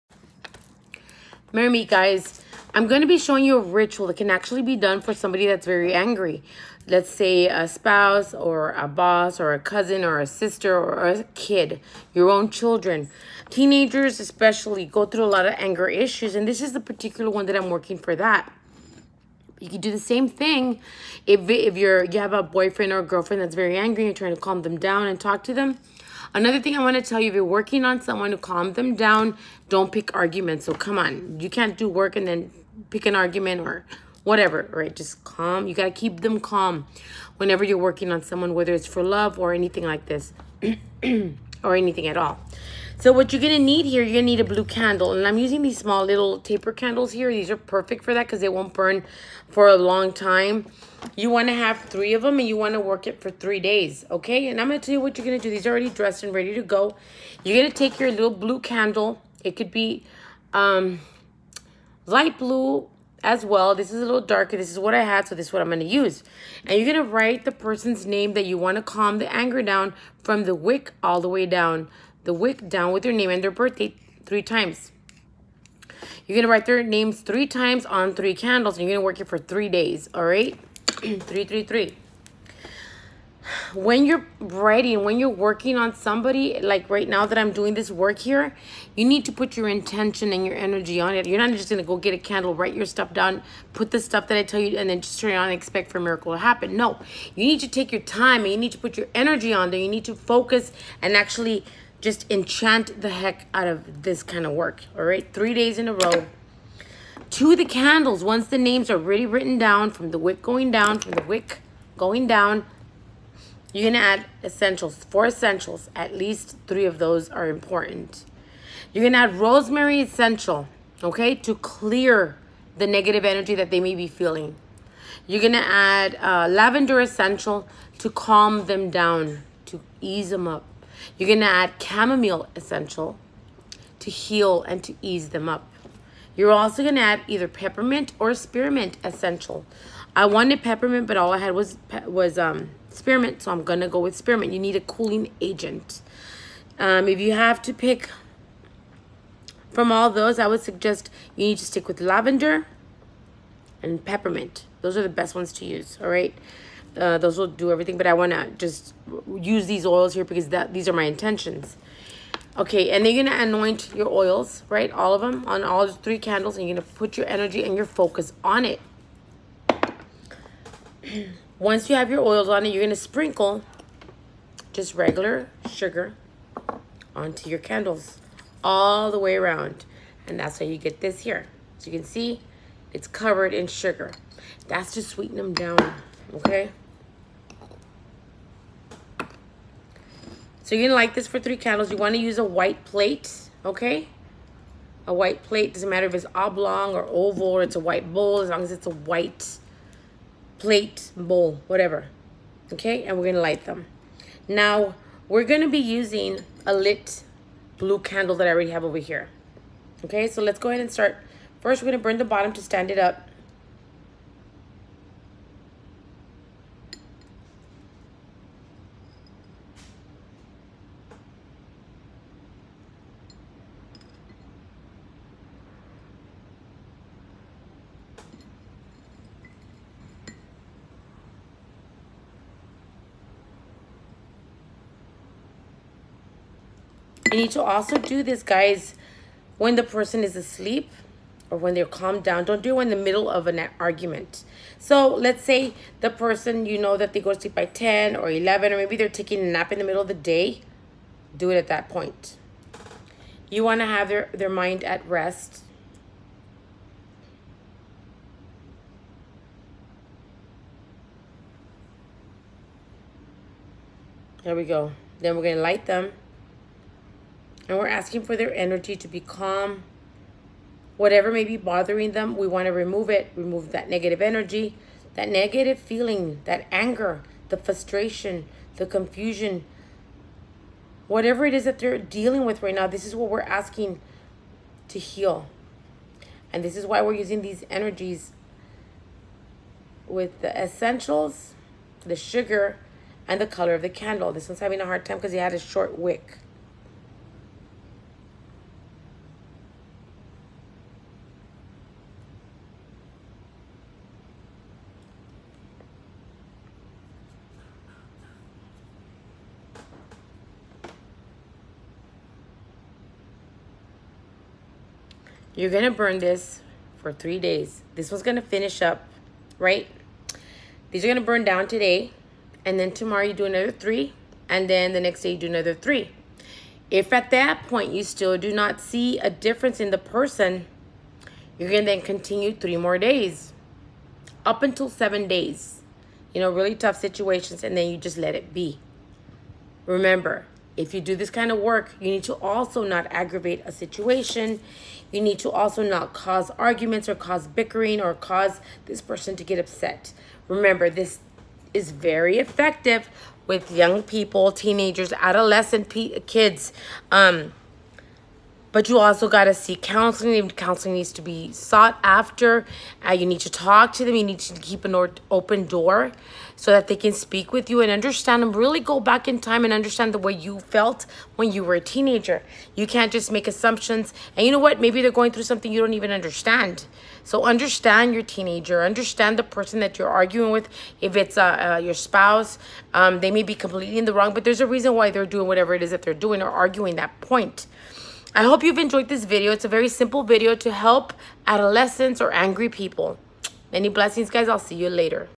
Calming Anger in Teens/ or Loved one
Here you going to need three taper blue candles, lavender essential, chamomile essential, peppermint essential, and rosemary essential if you need to contact me you can at Or visit our shop EnchantedBellewitch